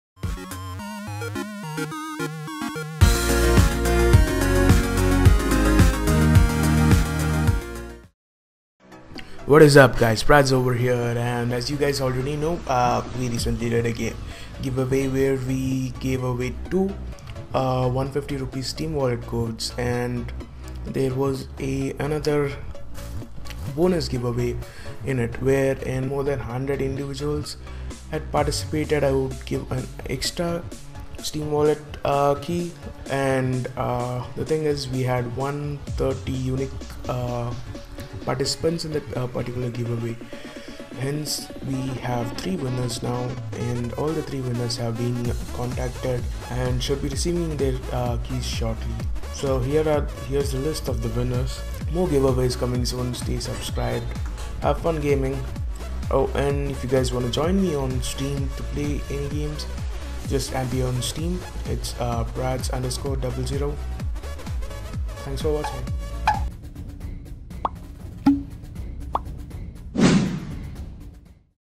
STEAM WALLET CODE GIVEAWAY WINNERS | 150rs Steam wallet code giveaway
the giveaway was a huge success thnx to all those who join in it
if u havent won it this time no need to worry more giveaways are coming soon including CSGO skins, CSGO game Keys and more wallet codes

till then have fun gaming